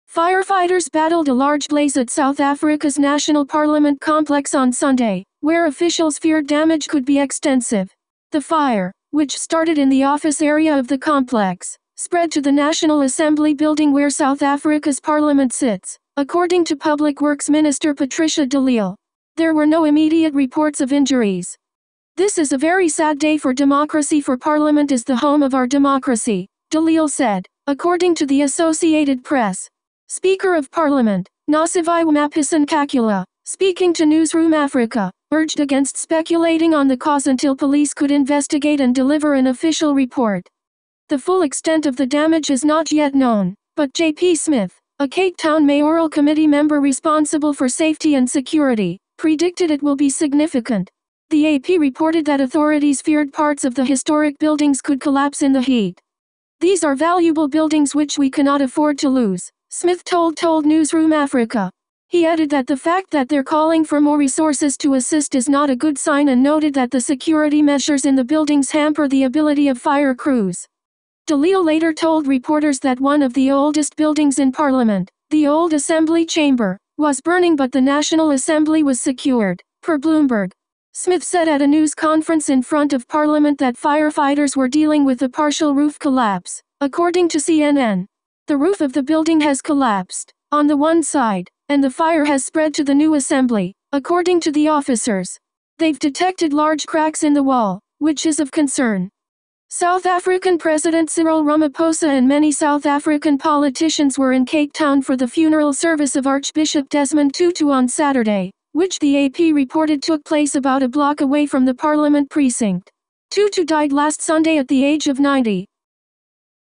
South African Fire sweeps through Parliament, damaging historic buildings
Firefighters battled a large blaze at South Africa’s national parliament complex on Sunday, where officials feared damage could be extensive.
The fire, which started in the office area of the complex, spread to the National Assembly building where South Africa’s Parliament sits, according to Public Works Minister Patricia de Lille. There were no immediate reports of injuries.

This is a very sad day for democracy for Parliament is the home of our democracy,” de Lille said, according to the Associated Press.
Speaker of Parliament, Nosiviwe Mapisa-Nqakula, speaking to Newzroom Africa, urged against speculating on the cause until police could investigate and deliver an official report.

The full extent of the damage is not yet known, but JP Smith, a Cape Town mayoral committee member responsible for safety and security, predicted it "will be significant.” The AP reported that authorities feared parts of the historic buildings could collapse in the heat.

"These are valuable buildings which we cannot afford to lose,” Smith told told Newzroom Africa. He added that “the fact that they’re calling for more resources to assist is not a good sign” and noted that the security measures in the buildings hamper the ability of fire crews.

De Lille later told reporters that one of the oldest buildings in parliament – the Old Assembly Chamber – was burning but the National Assembly was secured, per Bloomberg.
Smith said at a news conference in front of parliament that firefighters were dealing with a partial roof collapse, according to CNN. “The roof of the building has collapsed, on the one side, and the fire has spread to the New Assembly, according to the officers. They’ve detected large cracks in the wall, which is of concern."

South African President Cyril Ramaphosa and many South African politicians were in Cape Town for the funeral service of Archbishop Desmond Tutu on Saturday, which the AP reported took place about a block away from the Parliament precinct. Tutu died last Sunday at the age of 90.